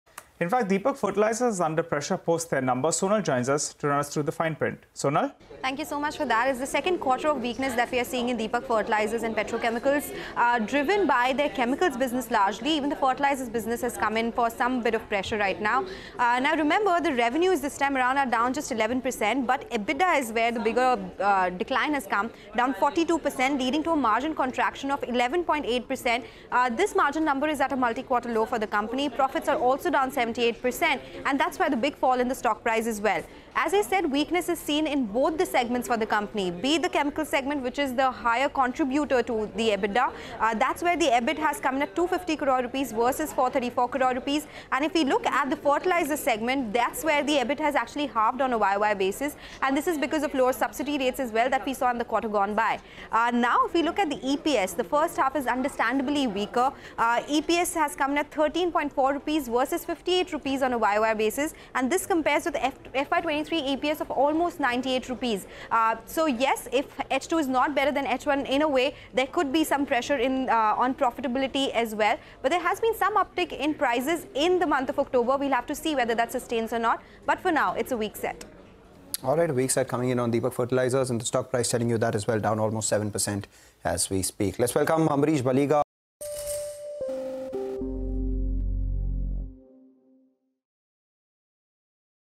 Deepak Fertilisers Q2: Revenue Down 11%, Profit After Tax Slumps 78% YoY | CNBC TV18
2QWithCNBCTV18 | Deepak Fertilisers is under pressure after reporting a weak set of Q2FY24 numbers. Revenue declines 11% and profit After Tax slumps 78% YoY. Sonal Bhutra with the details.

n18oc_business